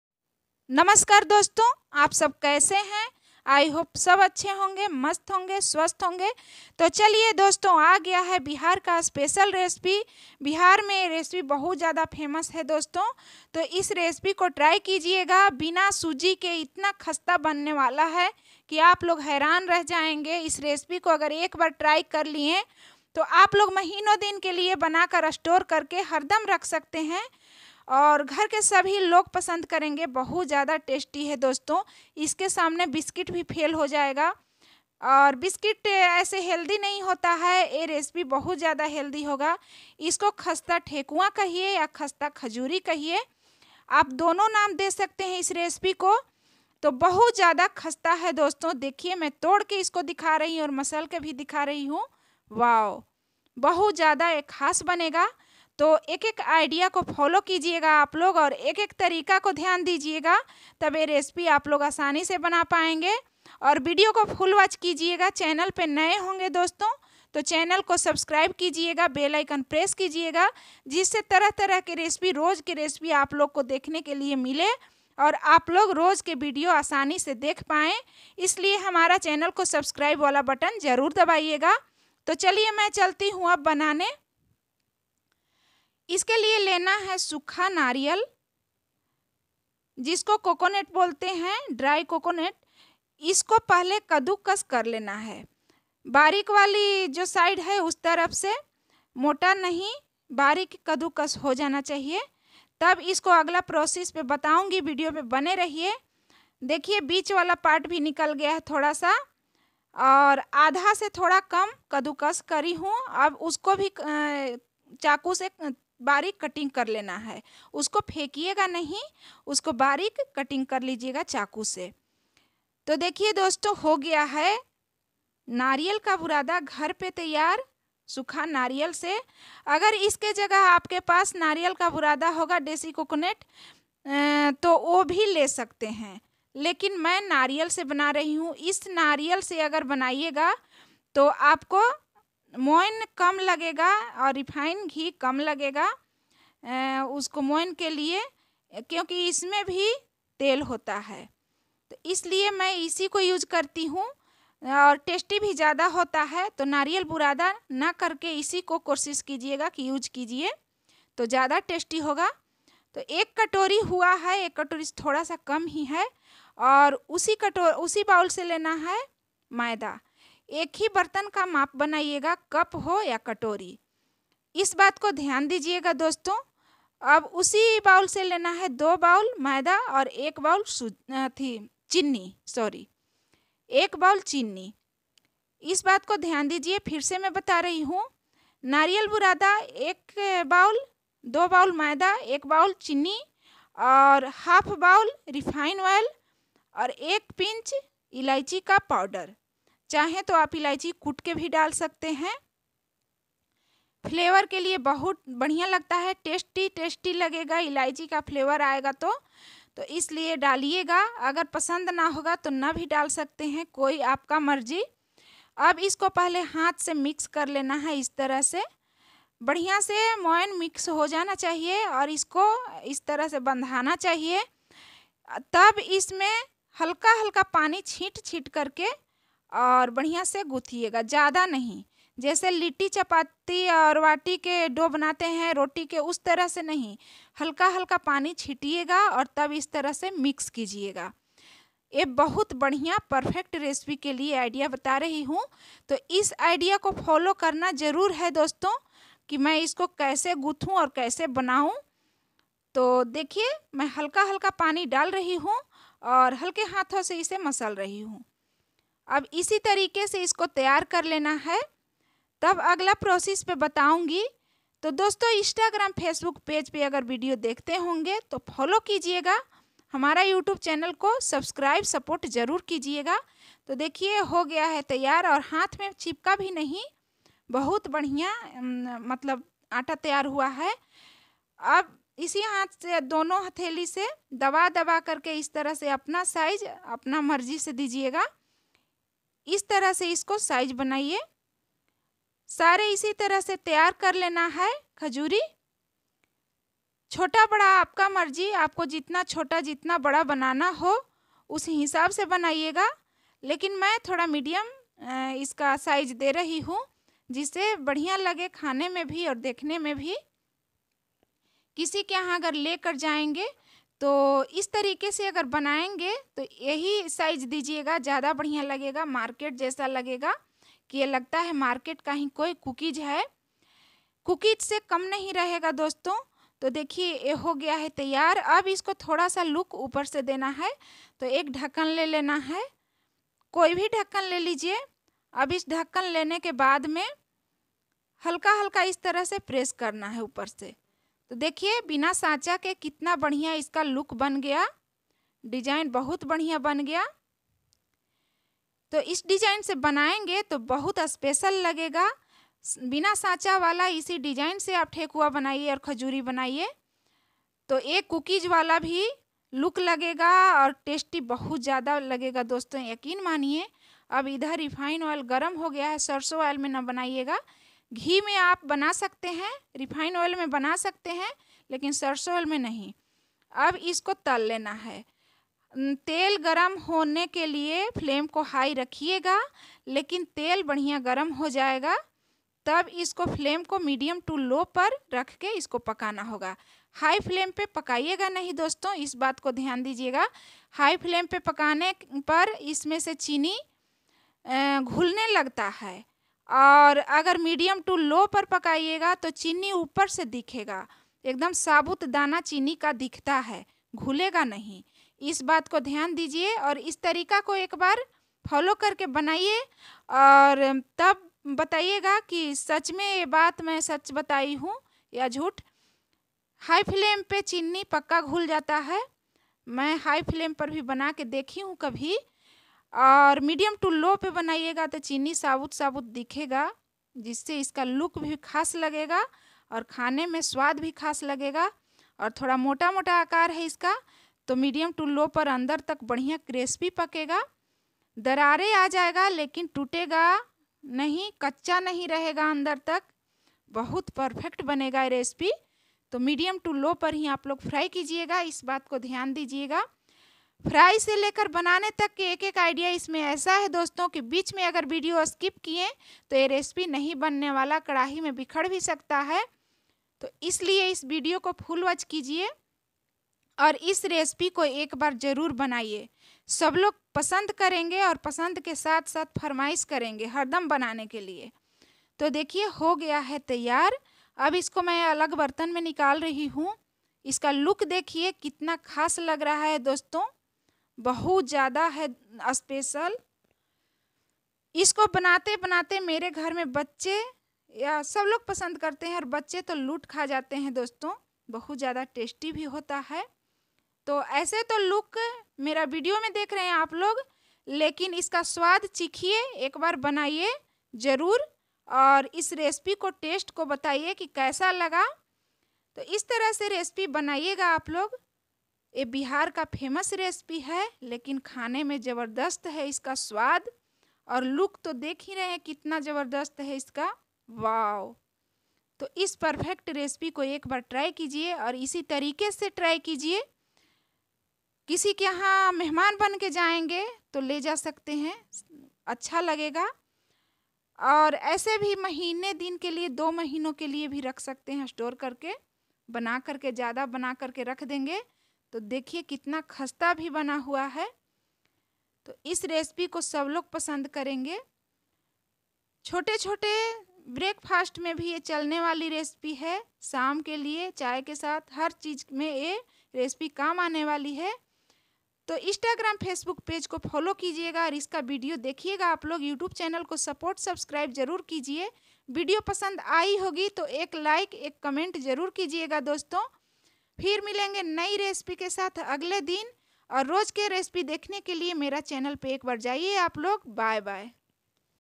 नहीं सूजी नहीं घी,कुकीज के स्वाद में बनाइए खस्ता खजूरी,बिहारी ठेकुआ भी कह सकते हैं🤤👌।khasta khajuria
Ingredients ➡️

1, cup kaddu kas Kiya hua Sukha nariyal

2, cup maida

1, cup chinni

1/2, cup refine oil

1, pincy elaichi powder

fry ke liye 200 gram refiner oil